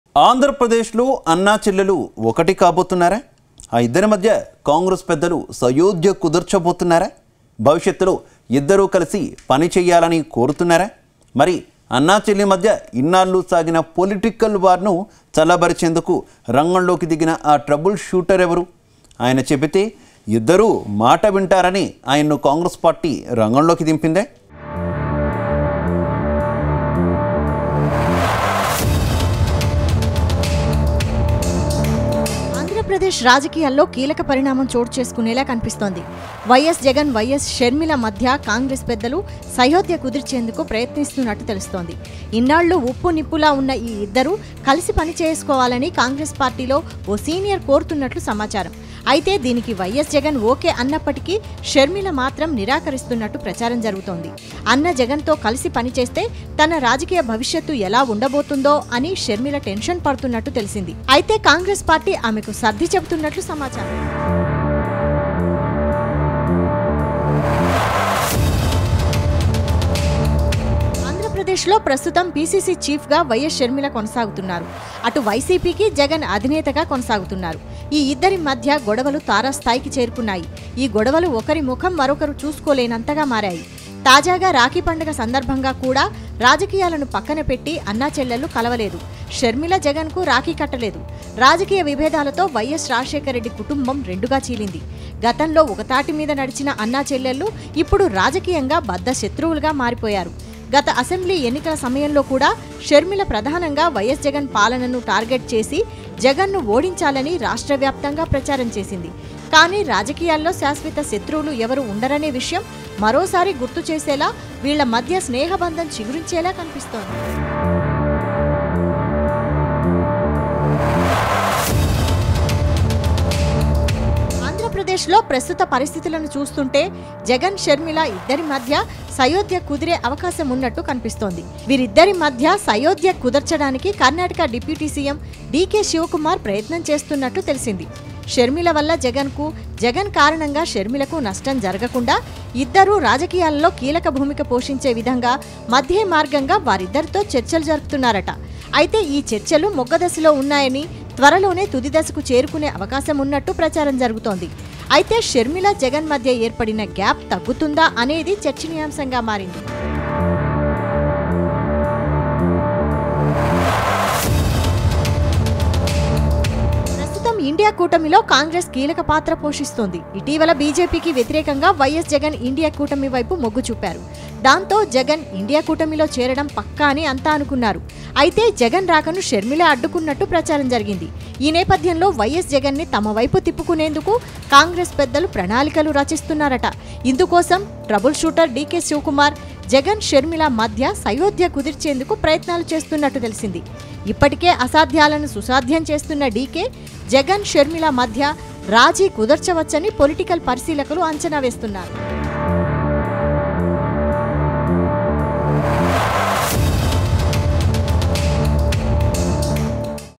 Sharmila,YS Jagan Trouble Shooter DK Shiva Kumar | అన్న, చెల్లెను కలిపేందుకు రంగంలోకి ట్రబుల్ షూటర్
YS Sharmila , YS Jagan Trouble Shooter D.K | అన్న, చెల్లెను కలిపేందుకు రంగంలోకి ట్రబుల్ షూటర్ | ZEE Telugu News